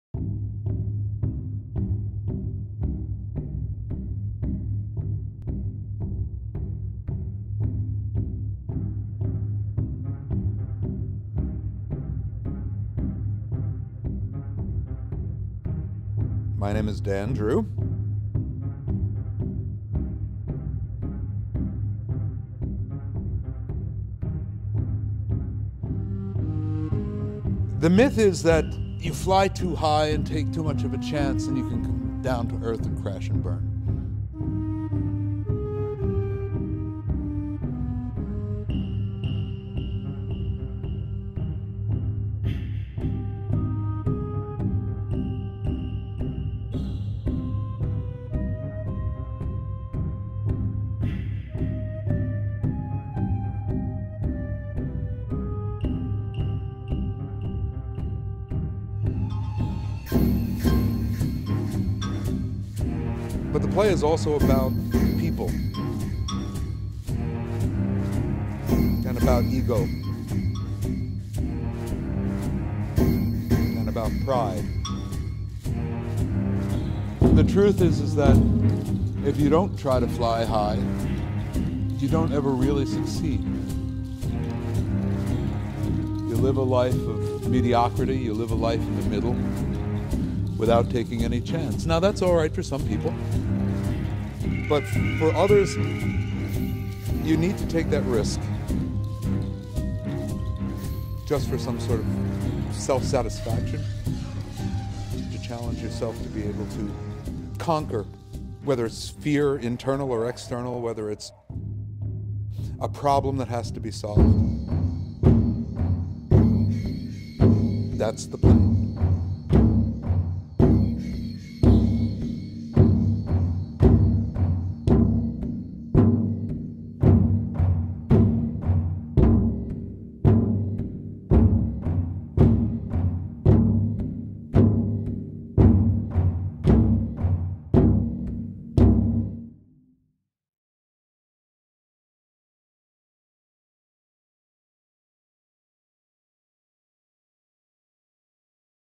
"I" (The Icarus Project) Teaser
"i" (The Icarus Project)
Adapted from the Myth
Written & Directed by Dan Drew

Jim Wise Theatre
Kupfrian Hall, NJIT

October 23-26 at 7pm
October 27 at 2:30

$12 Regular Admission
$10 Students & Seniors

Video shot by Lea Burlew, Edited by Rebecca Cortes